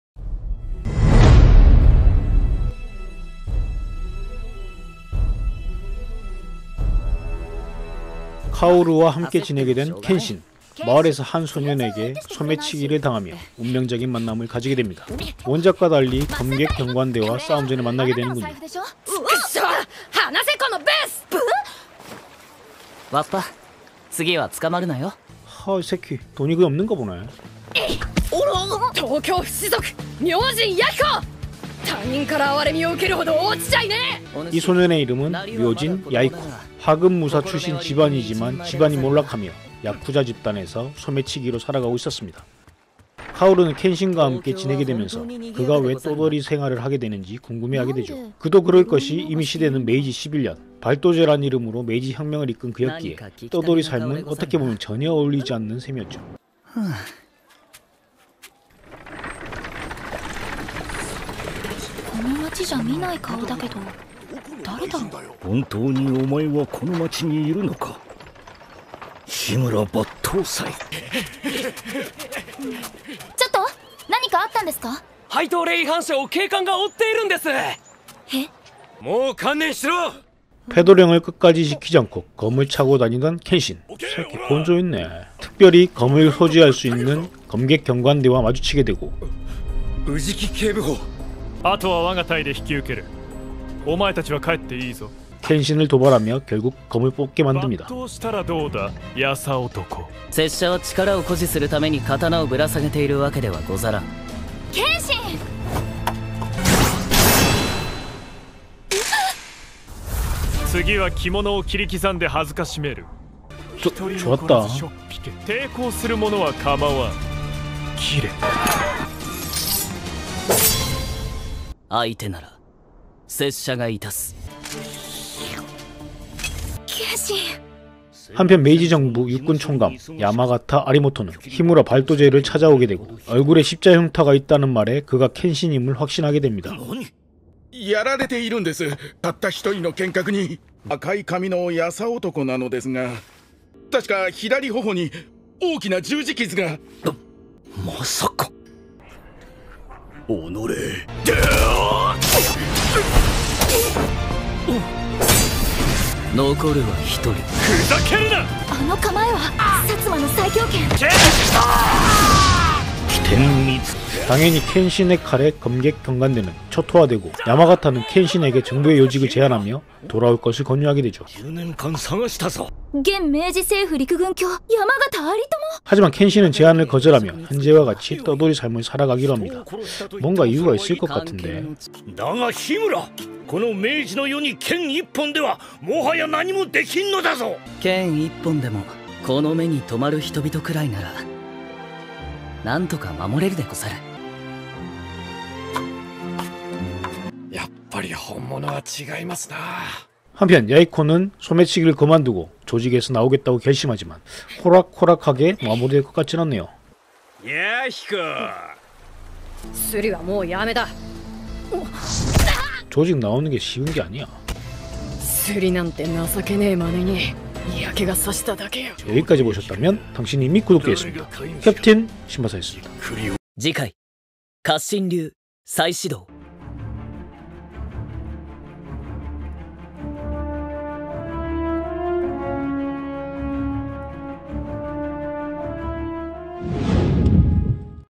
묘진 야히코|바람의 검심 2화 리뷰
카미야 활심류에 없어선 안되는 인물이죠!
반항적인 눈빛은 정말 잘 표현했군요 ㅎㅎ

깊이가 다른 애니리뷰채널 Channel 캡틴 심바사!